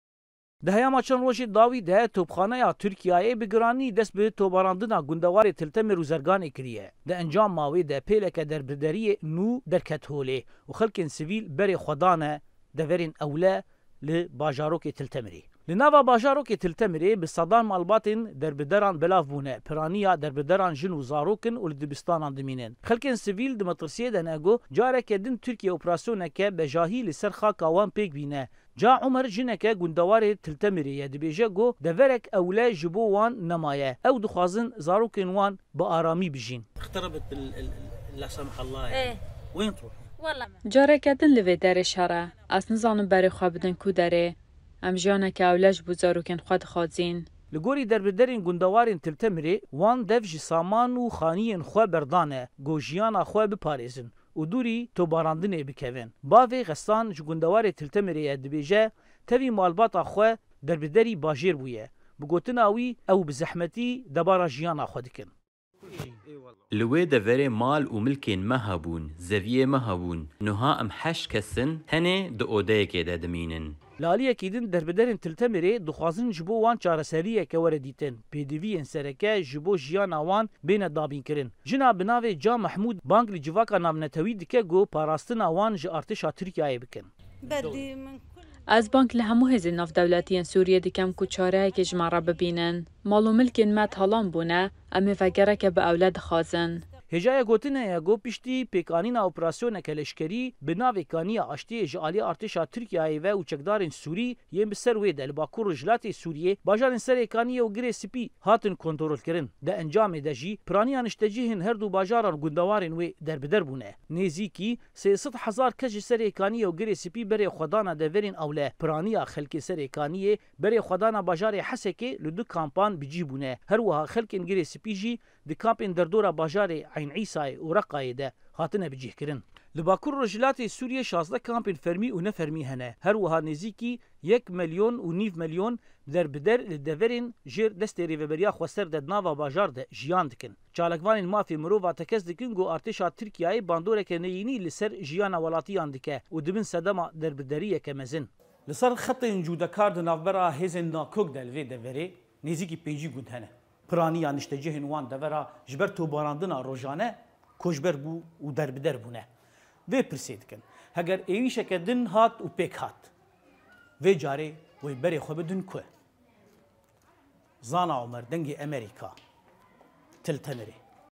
Topbarana Tirkîye li Surîye Xelkê Heremê Derbeder Dike | Dengê Amerîka
Ji ber topbarana Tirkiye li Bakur û Rojhelatê Suriye, ji malen xwe direnin dibin derbeder. Zana Omar raporteke nû li ser erîşen Tirkîye û derbederbûna xelkê heremê amede kiriye.